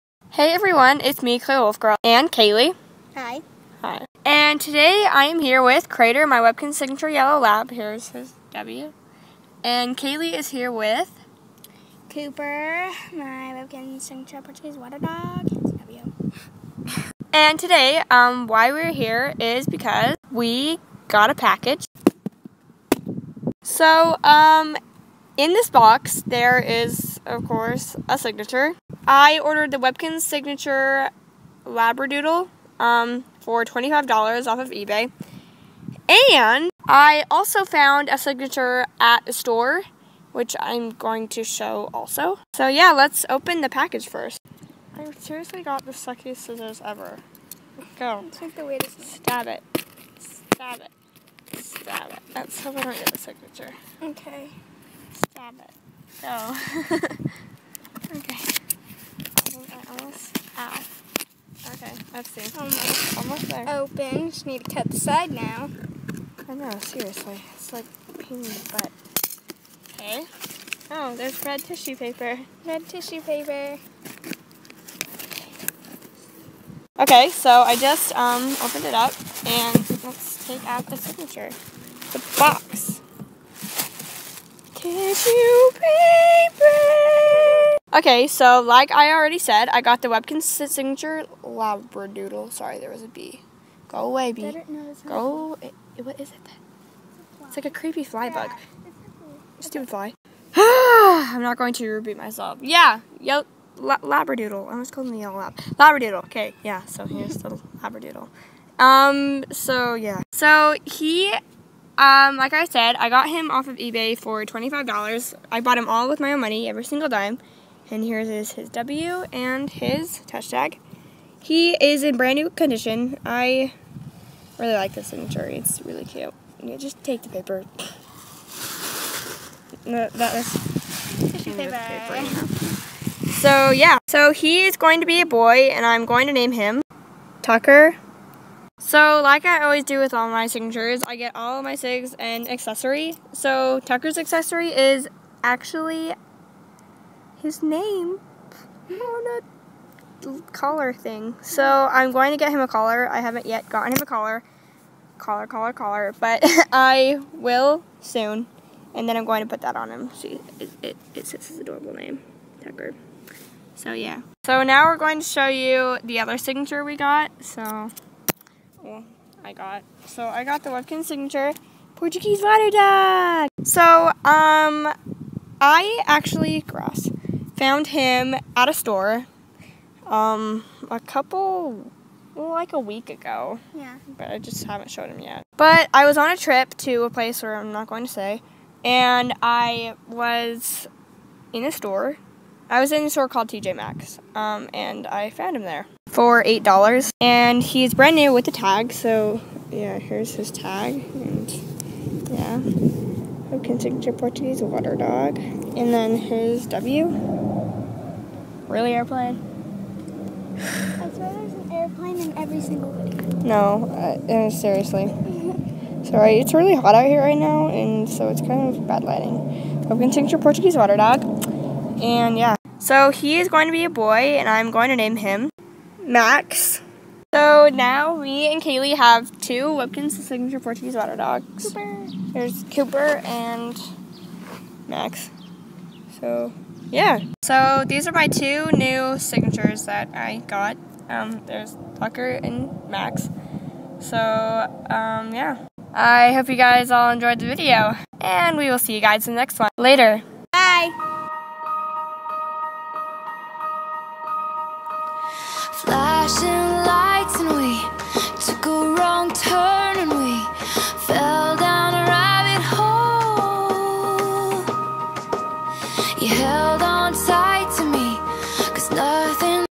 Opening the Webkinz Signature Labradoodle! (+ new Sig)
*This is not to brag or show off in anyway*
Hey everyone!
Today I got 2 new Signatures! The Signature Labradoodle and the Signature Portuguese Water Dog! The Labradoodle is a boy named Tucker! And the Portuguese water dog is also a boy named Max!